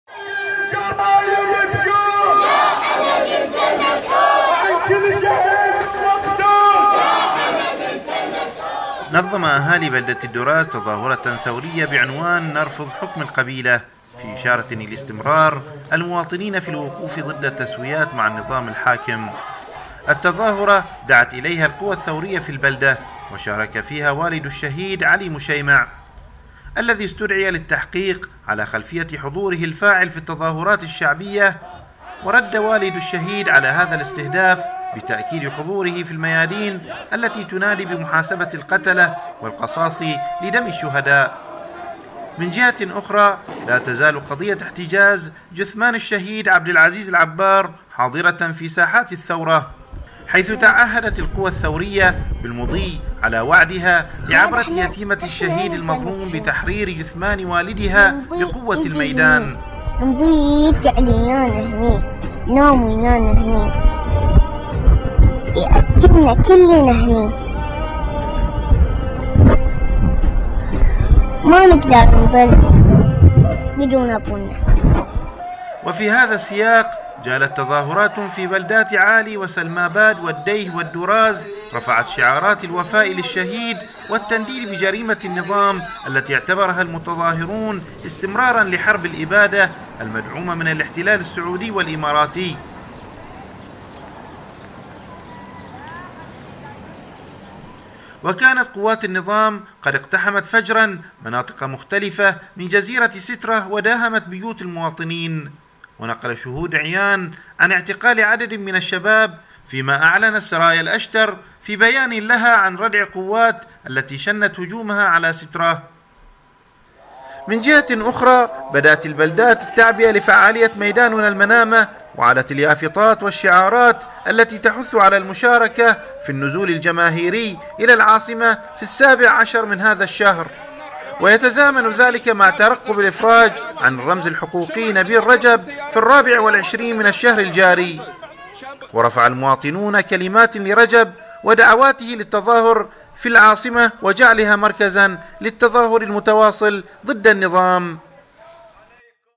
وكالة أنباء البحرين اليوم | ١٣ مايو ٢٠١٤ | الجماهير البحرانية ترفض حكم القبيلة
نظّمَ أهالي بلدة الدّراز تظاهرةً ثوريّة بعنوان "نرفض حكم القبيلة" في إشارةٍ إلى استمرار المواطنين في الوقوفِ ضدّ التسويات مع النّظام الحاكم.

التّظاهرة دعت إليها القوى الثّوريّة في البلدة، وشاركَ فيها والد الشّهيد علي مشيمع، الذي أستدعي للتّحقيق على خلفية حضوره الفاعل في التظاهرات الشّعبيّة، وردّ والد الشّهيد على هذا الاستهداف بتأكيد حضوره في الميادين التي تُنادي بمحاسبة القتلة والقصاص لدم الشّهداء.

من جهةٍ أخرى لا تزال قضيّة احتجاز جثمان الشهيد عبد العزيز العبّار حاضرةً في ساحات الثّورة، حيث تعهّدت القوى الثّوريّة بالمضي على وعدها لعَبْرة يتيمة الشهيد المظلوم بتحريرِ جثمان والدها بقوّة الميدان. وفي هذا السّياق، جالت تظاهرات في بلدات عالي وسلماباد والديه والدراز رفعت شعارات الوفاء للشّهيد والتّنديد بجريمة النّظام التي اعتبرها المتظاهرون استمراراً لحرب الإبادة المدعومة من الاحتلال السّعودي والإماراتيّ.

وكانت قوات النظام قد اقتحمت فجرا مناطق مختلفة من جزيرة سترة وداهمت بيوت المواطنين، ونقل شهود عيان عن اعتقال عدد من الشّباب، فيما أعلنت سرايا الأشتر في بيانٍ لها عن ردْع القوّات التي شنّت هجومها على سترة.

من جهةٍ أخرى، بدأت البلدات التعبئة لفعاليّة "ميداننا المنامة" وعلت اليافطات والشّعارات التي تحثّ على المشاركة في النزول الجماهيري إلى العاصمة في السابع عشر من هذا الشهر، ويتزامن ذلك مع ترقّب الإفراج عن الرّمز الحقوقيّ نبيل رجب في الرابع والعشرين من الشهر الجاري، ورفع المواطنون كلمات لرجب ودعواته للتظاهر في العاصمة وجعْلها مركزاً للتظاهر المتواصل ضدّ النّظام.